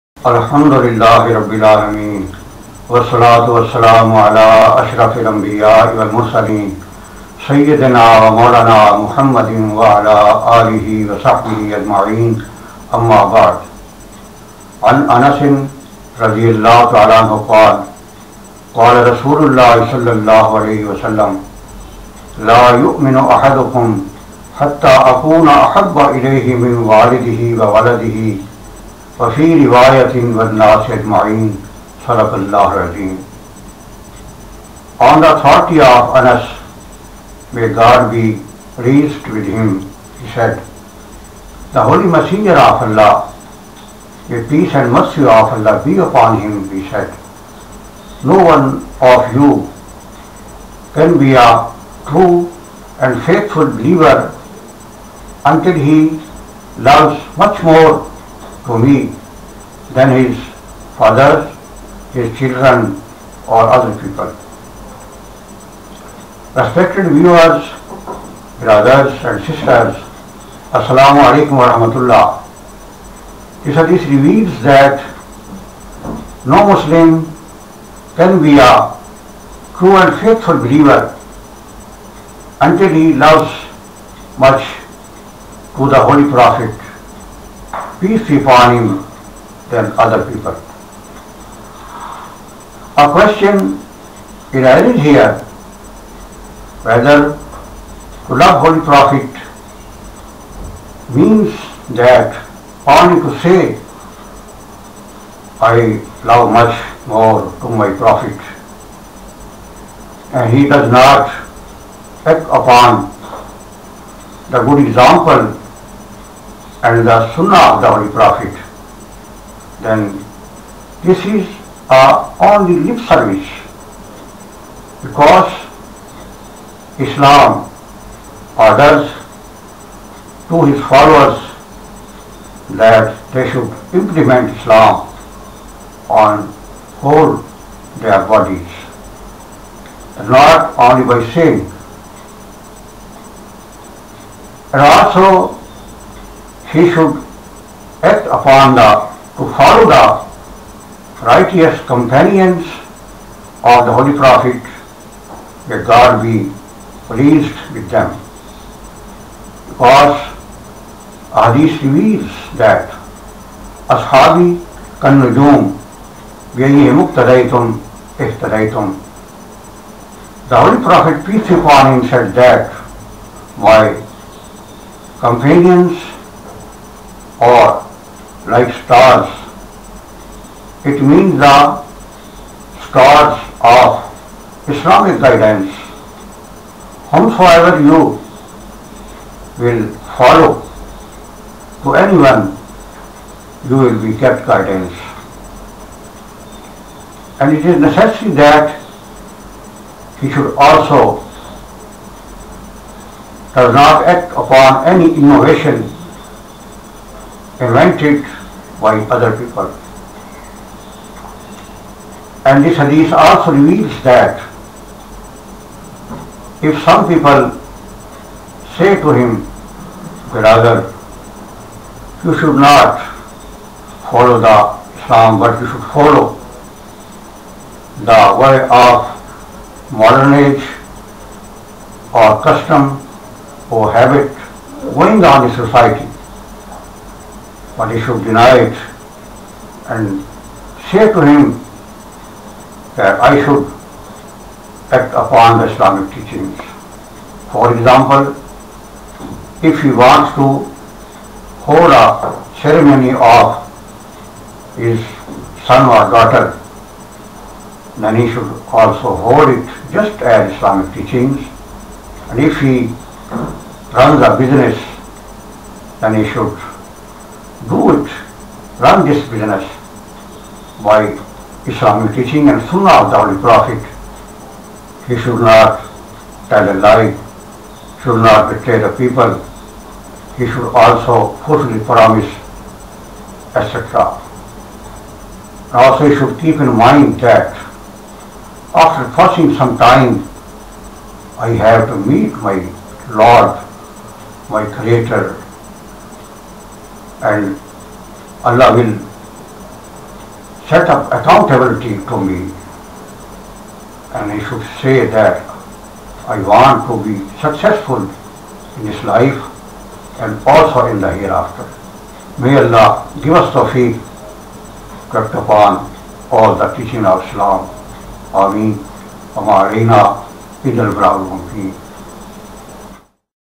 Commentary on a Hadith of Bukhari and Muslimby Al-Huda Islamic Videos
Respected viewers, this hadith reveals that none of a Muslim can be a true and faithful believer till the holy Prophet PBUH is dearer to him then his father, child and other mankind, Must watch.

Most viewed on this channel